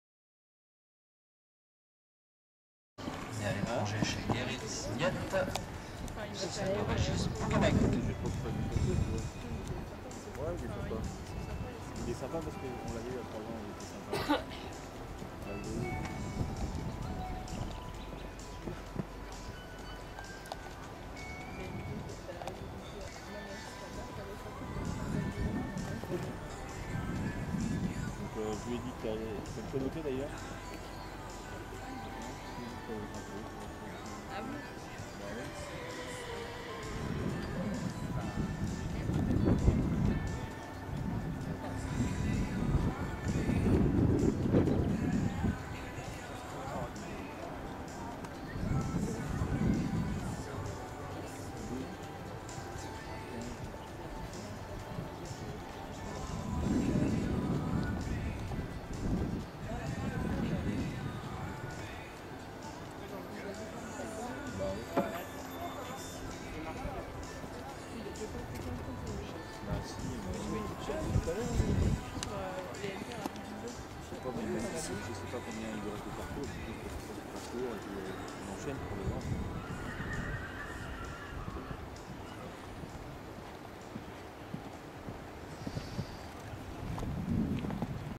Elesse MG (President)
Video details Elesse MG is by President (KWPN) and Poesie de Fallogne by Wiensender

♂  president (KWPN - Gris - 0 m - 1997):

●      Perdormances:

●      Production: Il a notamment produit Tonton Van Sint-Maarten étalon vainqueur de l'épreuve réservée aux étalons de 7 ans lors de l'approbation SBS, Der Senaat 111 étalon, Don Quichotte VD Kolmen finaliste du Championnat de France des 6 ans avec Stéphane Delaveau...